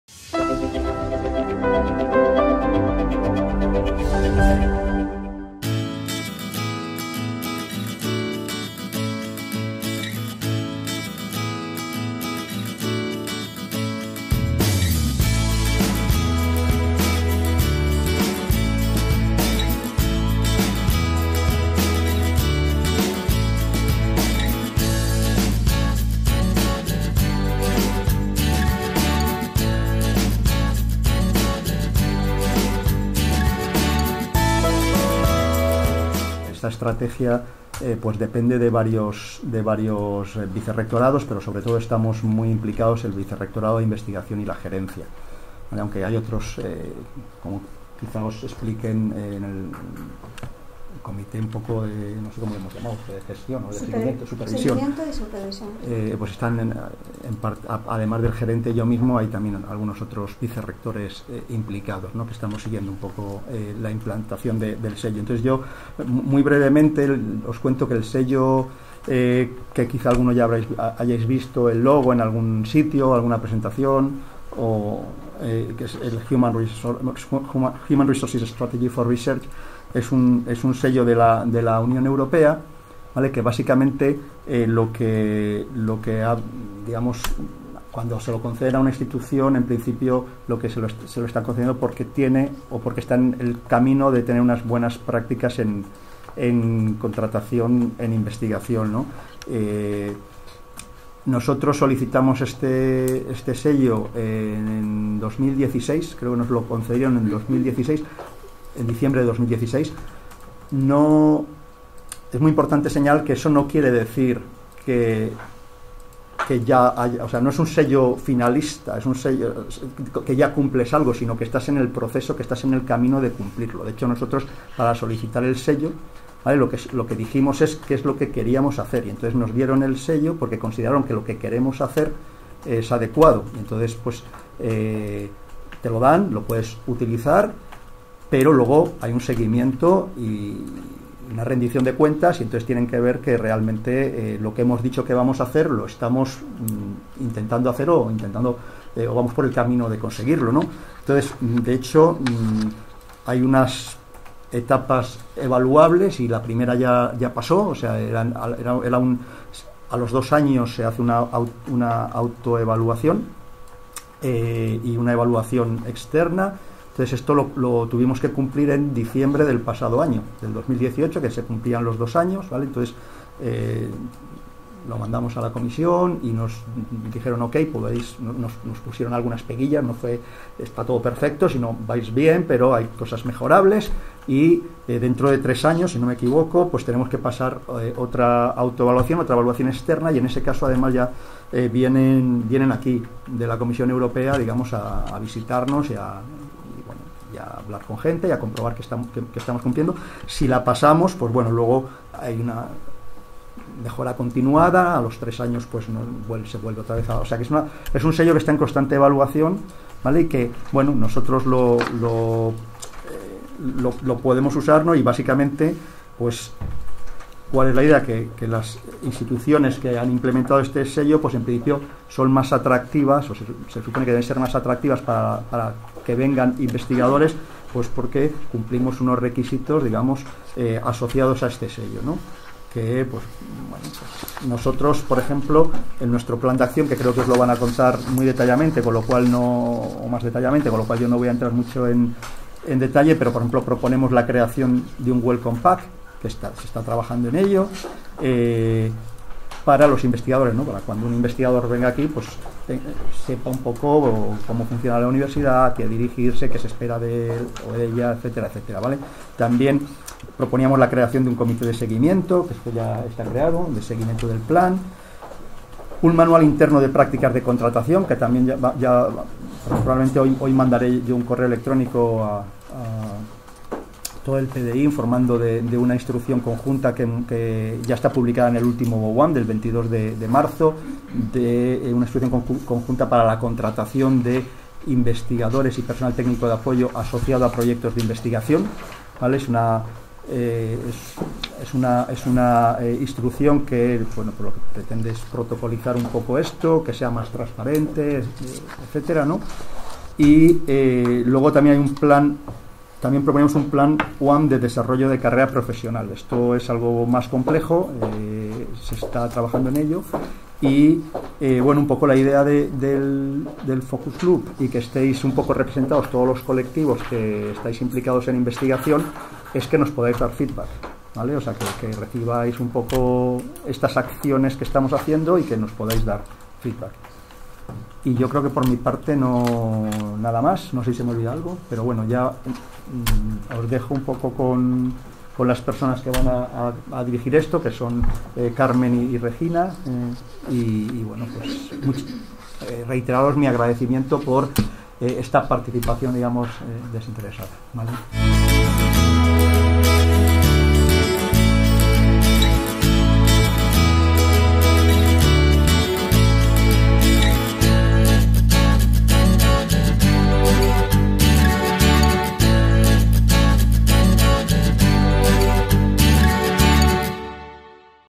La UAM crea un focus group para dar impulso a su Estrategia de Recursos Humanos para Investigadores
Representantes de los colectivos implicados en investigación participan en un grupo de trabajo sobre el sello de calidad europeo Human Resources Strategy for Research (HRS4R).
Guía de bienvenida para nuevos investigadores, Manual de buenas prácticas en la contratación del personal investigador, Plan de desarrollo de la carrera profesional y propuestas para impulsar la visibilidad de las investigadoras. Estos son algunos de los ejes incluidos en el Plan de Acción elaborado por la Universidad Autónoma de Madrid (UAM) para impulsar su Estrategia de Recursos Humanos para Investigadores (HRS4R, por sus siglas en inglés).

La UAM recibió en diciembre de 2016 el sello de calidad HRS4R, un distintivo que concede la Comisión Europea a universidades e instituciones que generan y apoyan la existencia de un entorno estimulante y favorable al trabajo de investigación.

Como parte de la implementación de la Estrategia, la UAM ha creado un focus group integrado por investigadores de las distintas categorías (desde Personal Investigador en Formación hasta catedráticos), así como máximos responsables del vicerrectorado de Investigación y de la Gerencia de la UAM, con el objetivo de analizar y obtener valoraciones, ideas y propuestas que permitan impulsar todas las acciones que se están desarrollando dentro del Plan de Acción.

El grupo de trabajo acaba de celebrar una primera reunión, en la que el vicerrector de Investigación, José Manuel Rodríguez Sancho, expresó su agradecimiento a todos los participantes por sumarse a un grupo de trabajo que debe permitir a la UAM seguir avanzando en la mejora de sus políticas de recursos humanos para investigadoras e investigadores. El vicerrector recordó además que el sello HRS4R no es un distintivo finalista, sino que está sometido a una evaluación constante. “Para solicitar el sello nosotros dijimos qué es lo que queríamos hacer; y nos lo dieron, porque consideraron que lo que queremos hacer es adecuado. Pero hay un seguimiento y una rendición de cuentas, para comprobar que vamos por el camino de conseguirlo”. Garantizar que así sea es objetivo prioritario de esta y otras iniciativas puestas en marcha por el Vicerrectorado y la Gerencia de la UAM.

Por otro lado, y según explicó el vicerrector de Innovación, “está en fase de elaboración un Plan de desarrollo de la carrera profesional”, cuya finalidad será informar a los investigadores que se incorporan a la UAM sobre los distintos pasos de la carrera investigadora dentro del marco legal de nuestro país; difundir en la web el desarrollo de la carrera investigadora en la UAM y orientarles sobre las posibilidades de empleo en el ámbito de la investigación fuera de la UAM, tanto en el sector privado como en el público.

También se está elaborando un Manual de gestión de gastos en los proyectos de investigación, para informar sobre el procedimiento para la gestión de fondos y gastos, así como una traducción al inglés de contratos, convocatorias, y en general todos los documentos de la página web. La directora del Área de Investigación y Transferencia, Regina García Beato; y el vicegerente de Recursos Humanos, José Matías Hernández Sánchez, explicaron cómo el nuevo Manual de gestión de gastos ha sido diseñado para aligerar la carga burocrática a la que han de hacer frente los investigadores, facilitando tareas críticas como la selección y contratación de personal de apoyo.

La HRS4R de la Comisión Europea abarca varios siguientes ejes, referidos a la ética en la práctica profesional; a criterios y transparencia en los procesos de selección y contratación; a las condiciones de trabajo y a la supervisión y formación. Este distintivo refleja el compromiso de la UAM en la mejora de su Estrategia de Recursos Humanos, alineada con los principios de la Carta Europea del Personal Investigador y el Código de Conducta para la Contratación del Personal Investigador.